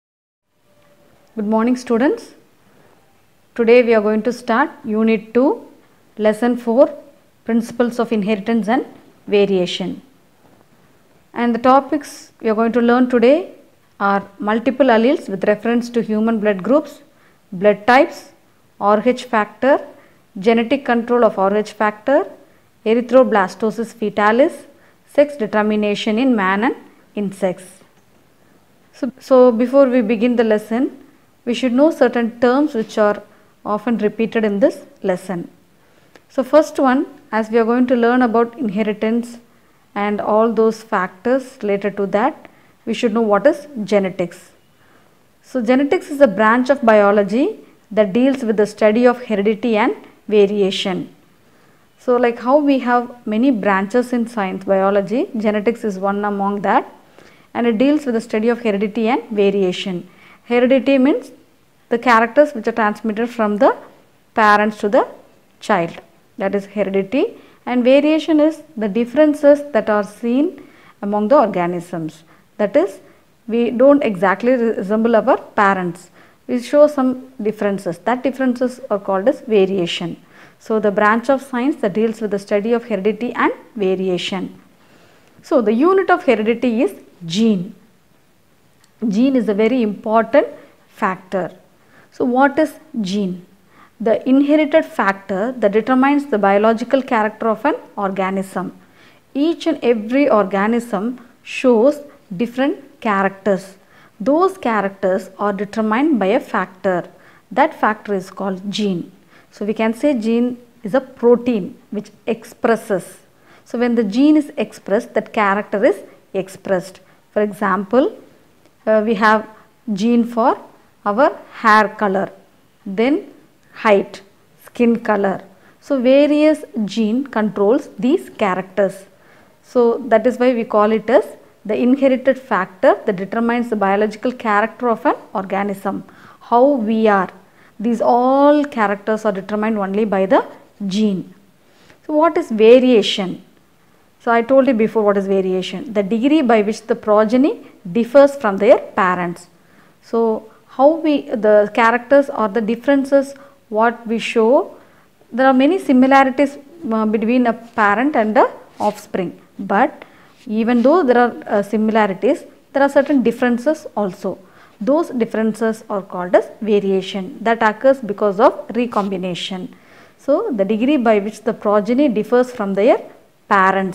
12 Zoology | Principles of Inheritance & Variation | Introduction | விலங்கியல் | Lesson 4 | Part 2
#+2 Bio
Lesson 4
Principles of Inheritance & Variation

Multiple Alleles
Human Blood Groups
ABO Blood Types
Rhesus or Rh Factor
Genetic Control of Rh Factor
Fisher & Race Hypothesis
Winer Hypothesis